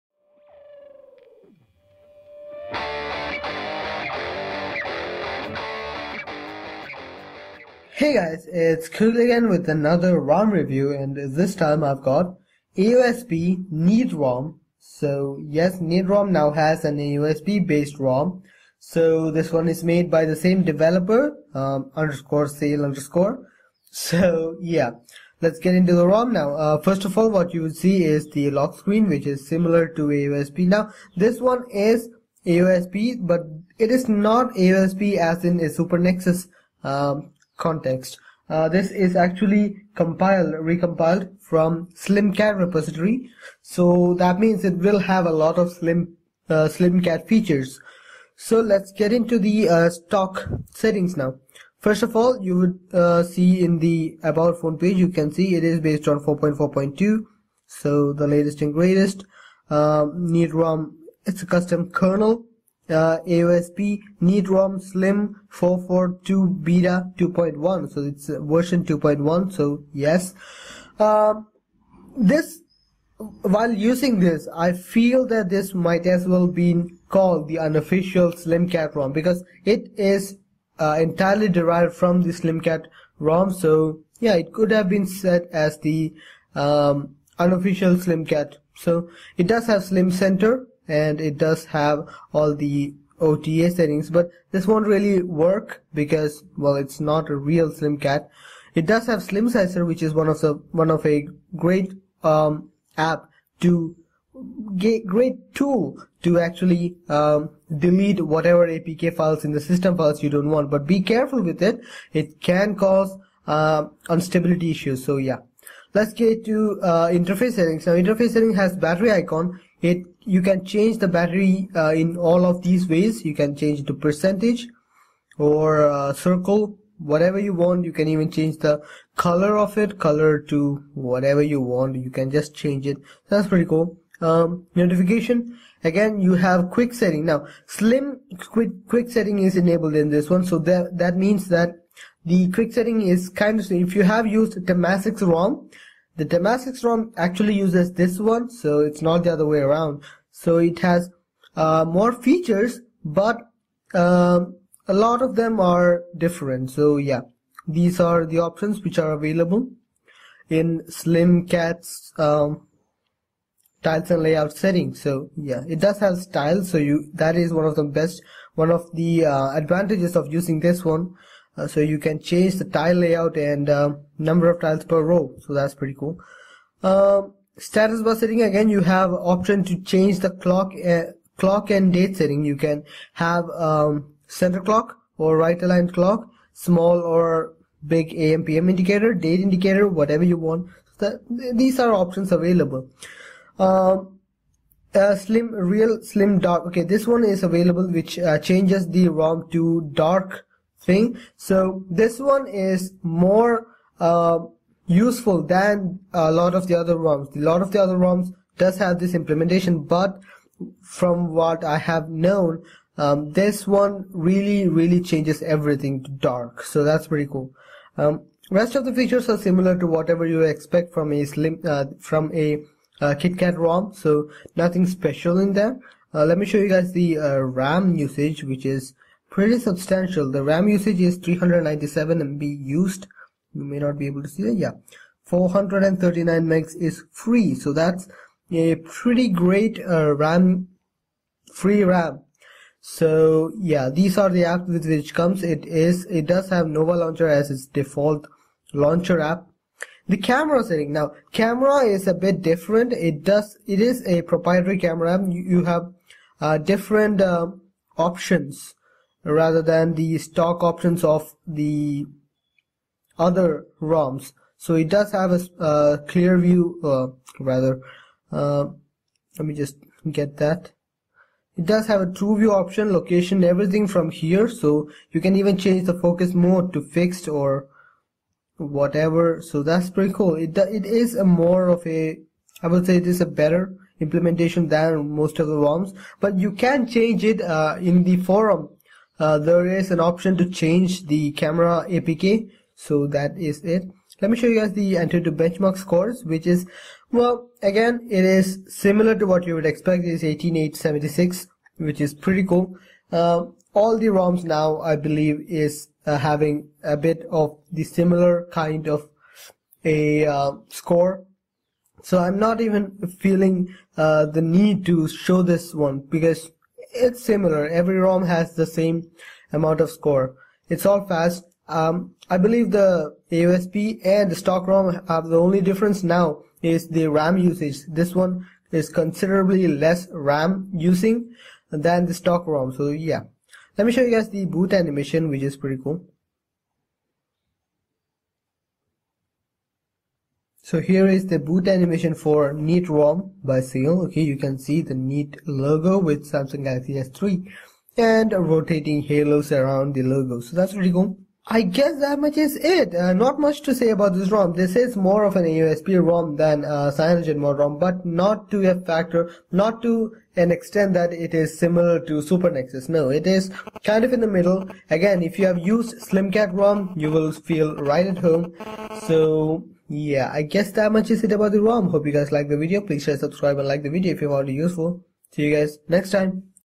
AOSP NeatROM (Android 4.4.2) For Galaxy S3 (GT-i9300)
_Sale_ Has Done it again and now he is foraging in to the AOSP World! with the AOSP compiled from slimKat Repository..